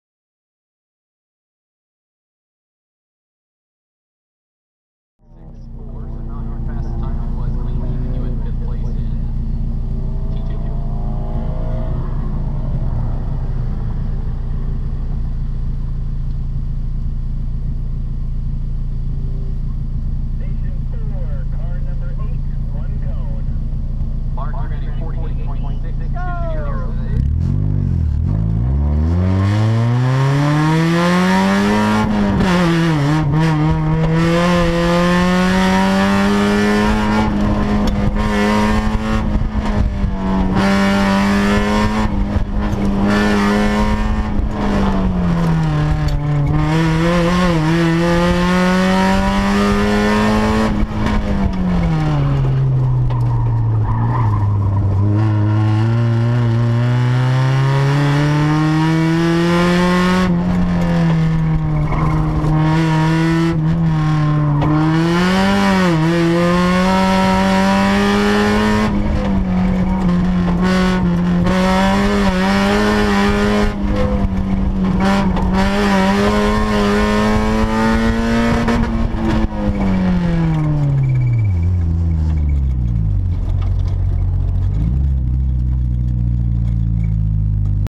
ASNW Autocross #13, Run 06
Autosports Northwest Autocross 13 in Deer Park, WA 09/14/14

1996 Mazda Miata "Cone Damage" ChumpCar